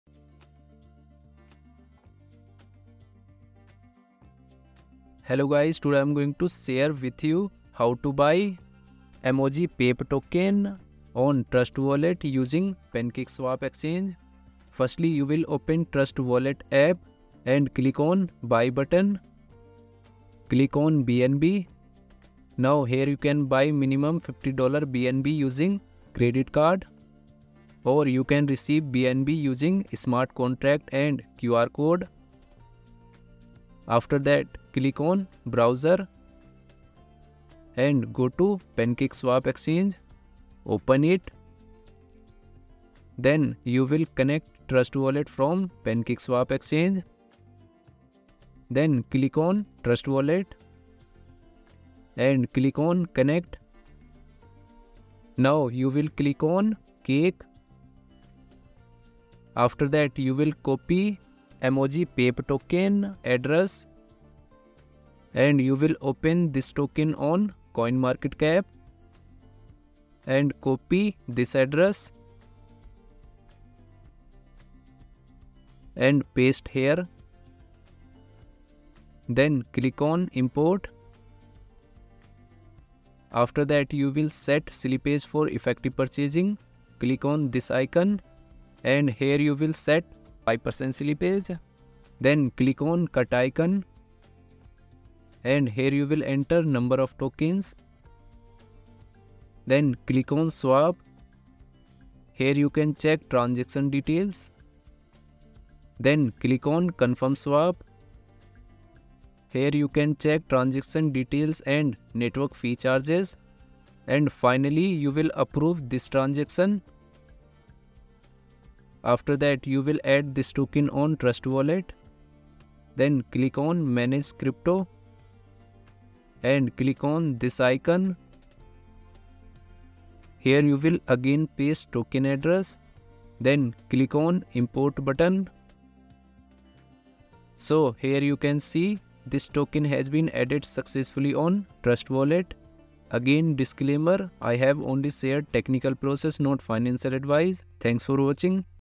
How to Buy MOG PEPE (PEPE) Token On Trust Wallet Using BNB and PancakeSwap Exchange
In this video, I will share with you where you can buy MOG PEPE (PEPE) Token using BNB and PancakeSwap Exchagne.